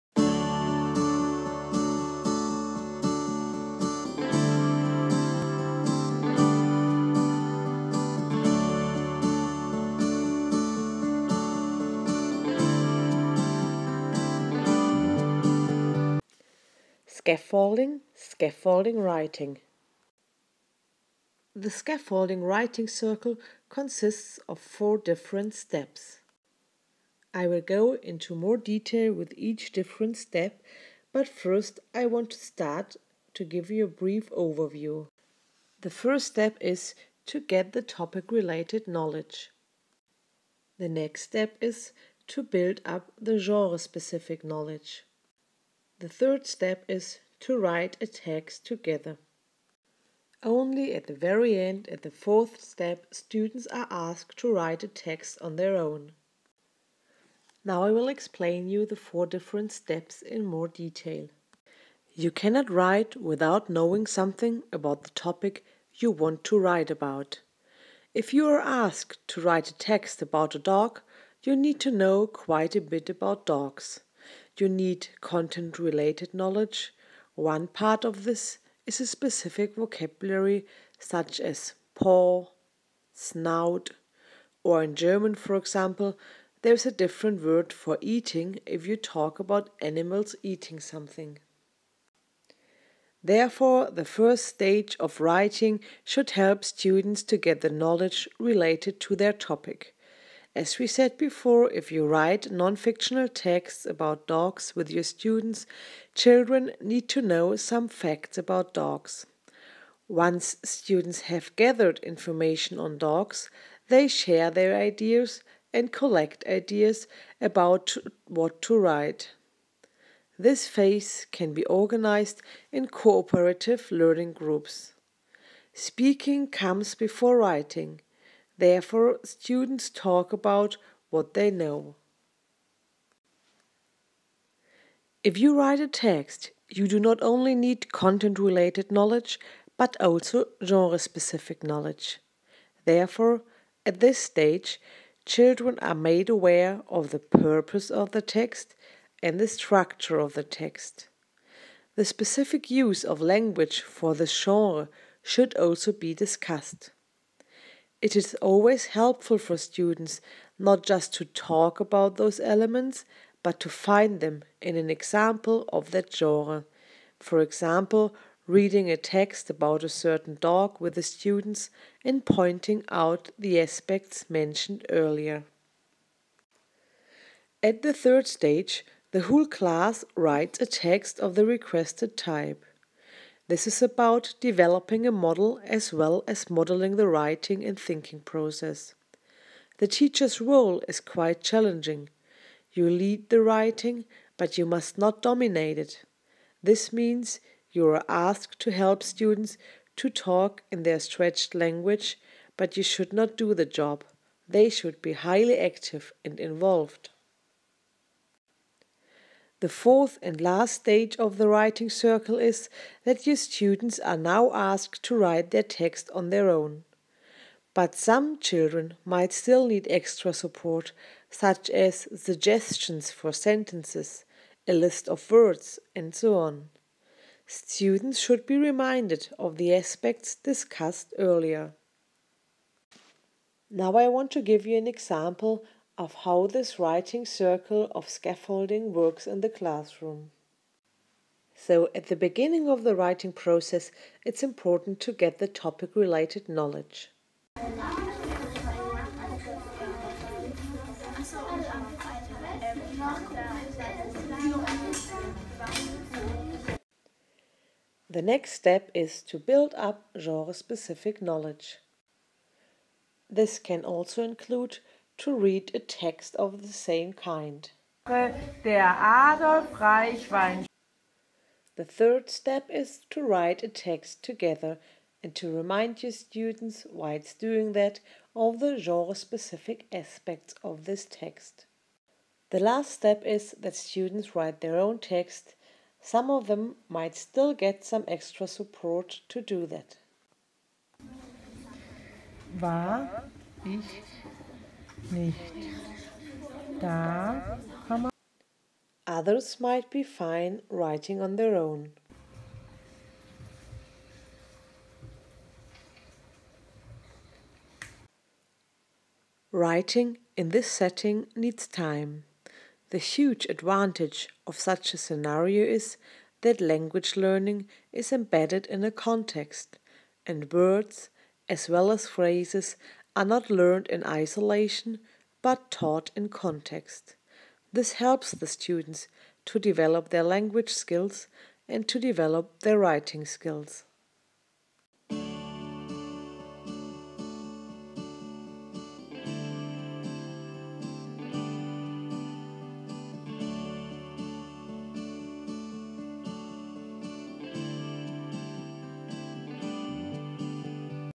The scaffolding circle for writing
In this video I explain the scaffolding circle of writing, consisting of 4 different steps.